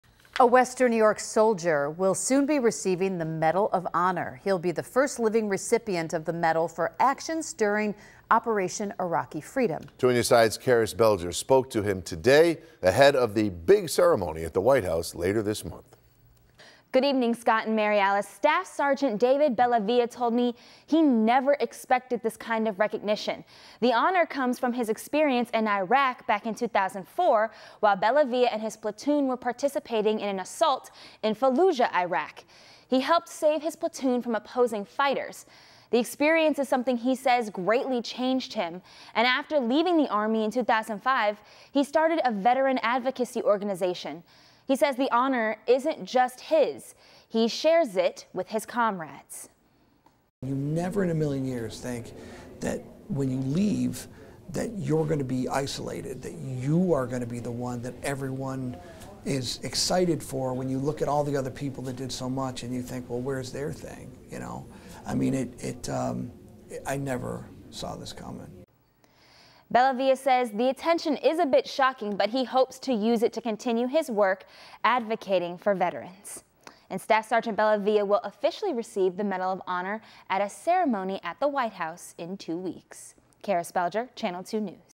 Local Soldier Receives The Medal Of Honor
Staff Sgt. Bellavia will officially recieve the Medal of Honor at a ceremony at the White House in two weeks